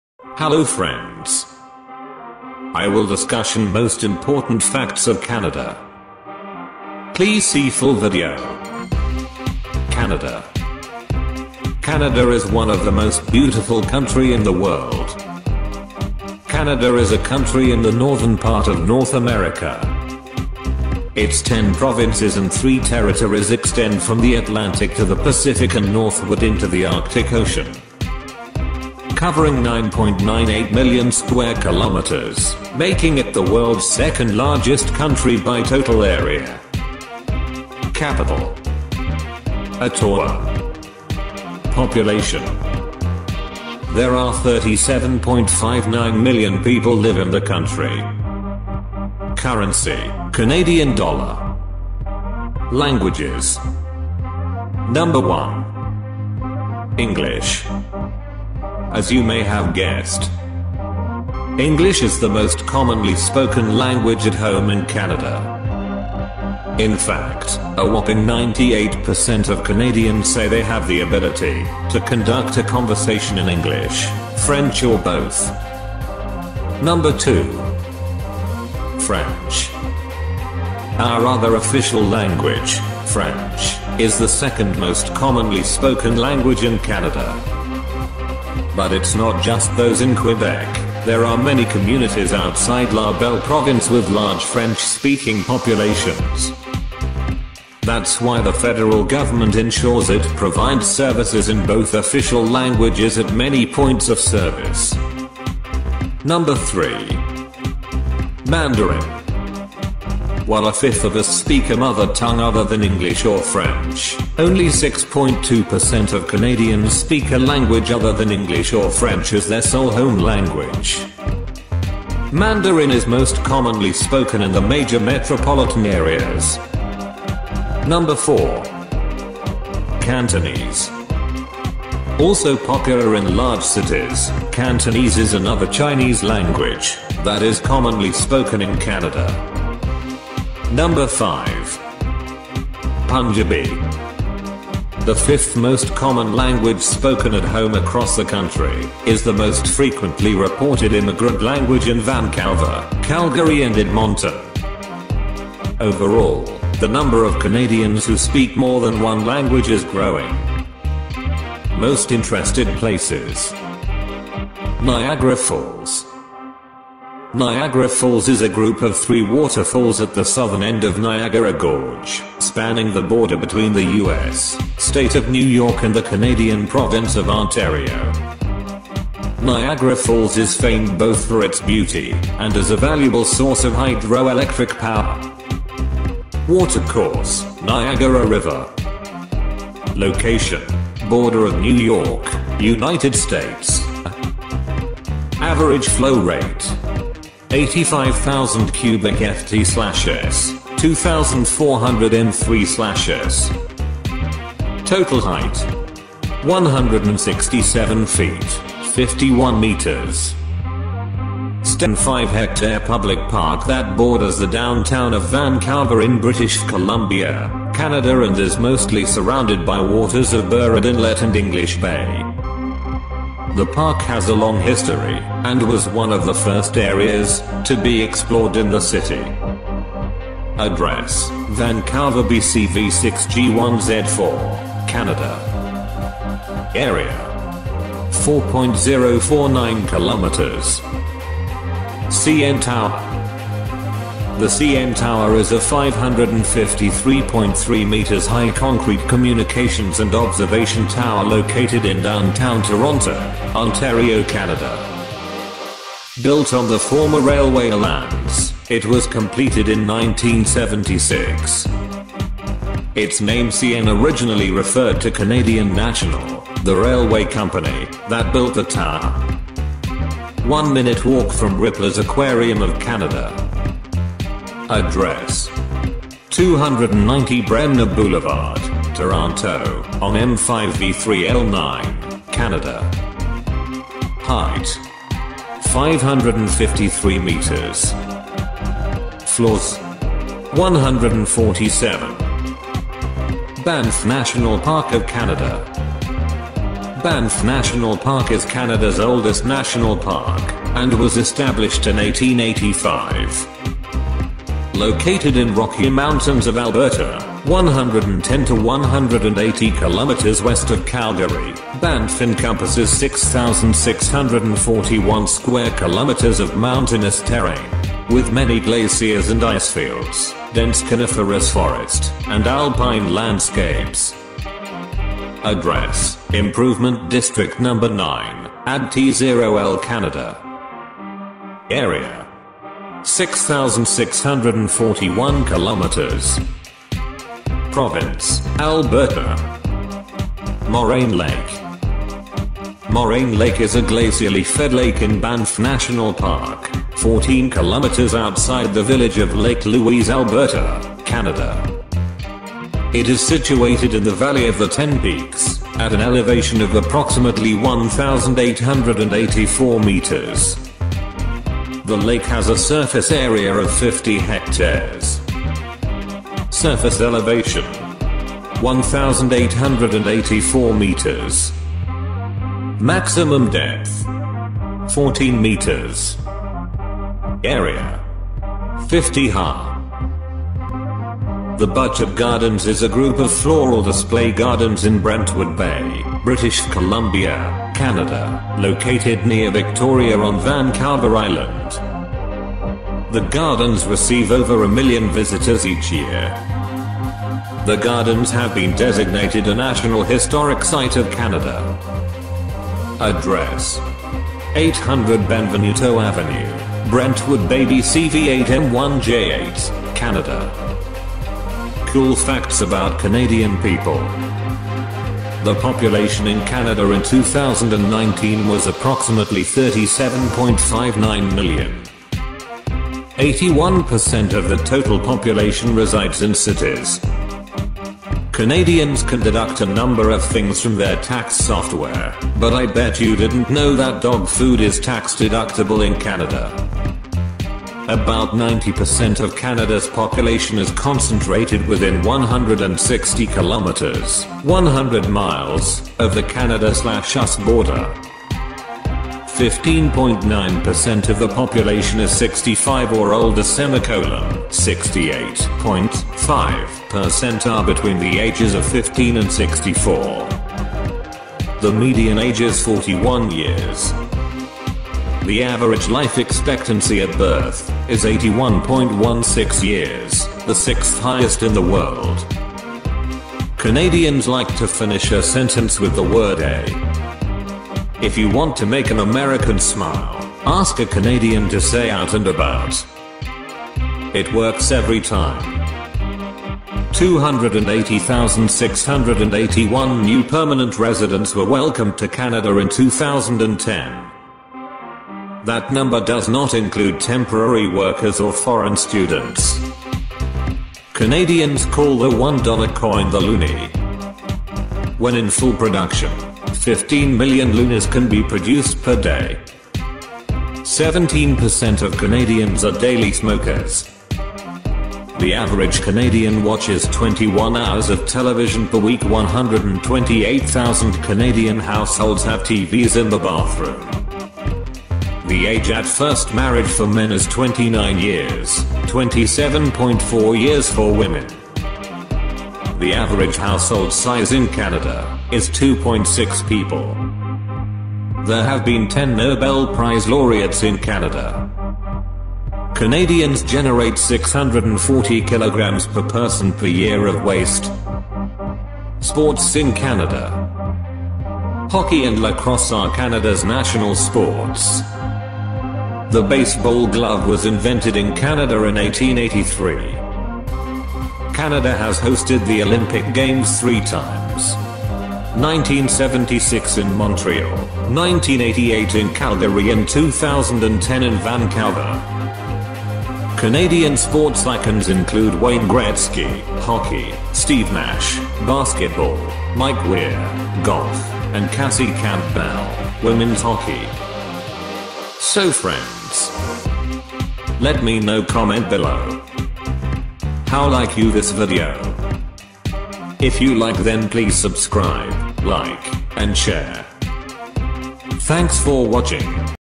Most Important Facts of Beautiful Canada
Today will show the most important facts of beautiful Canada. You will get all important information about Canada.

Credit:
Content: Various of Online website